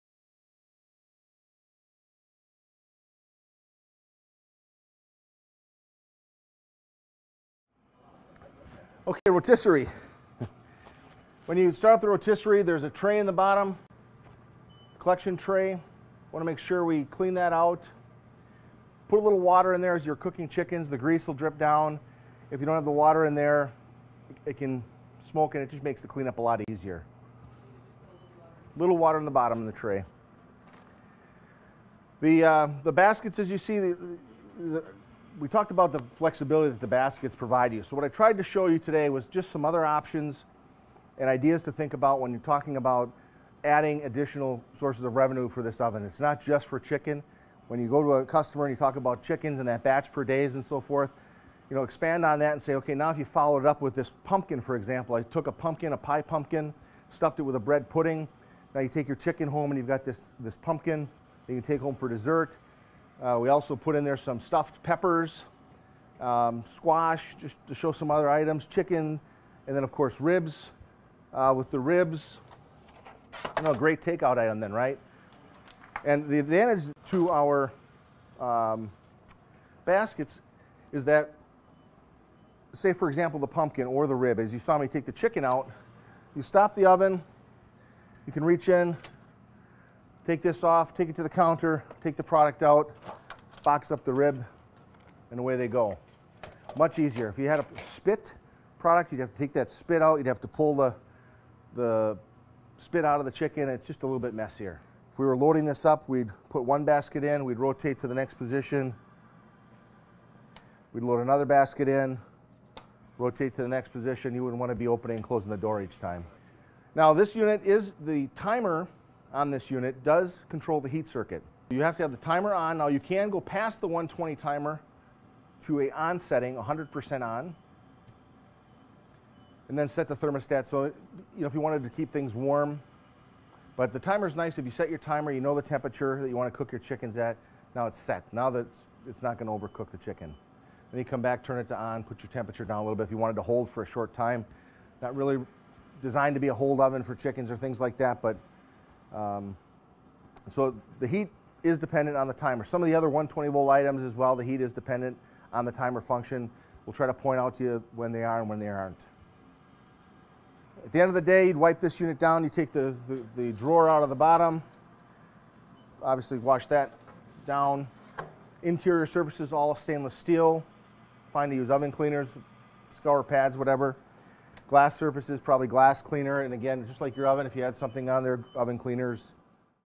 Vollrath Countertop Rotisserie Oven
The Vollrath countertop rotisserie oven can cook up to eight 3 lb. chickens in about 2 hours! Thanks to its glass front and rear, this countertop rotisserie oven is a great point of sale unit because it provides excellent product visibility. This countertop rotisserie oven is ideal for small convenience stores, delis, supermarkets, and other smaller locations. It's suitable for continuous use, and is even great for cooking ribs!

It features stainless steel construction with chrome plated wire baskets and a full size removable drip tray for easy cleanup. A specially-designed circulation fan ensures that the heat is evenly and consistently distributed for excellent cooking and browning. It is thermostatically controlled, has a 2700 watt heating element, and a 208/240V electrical rating.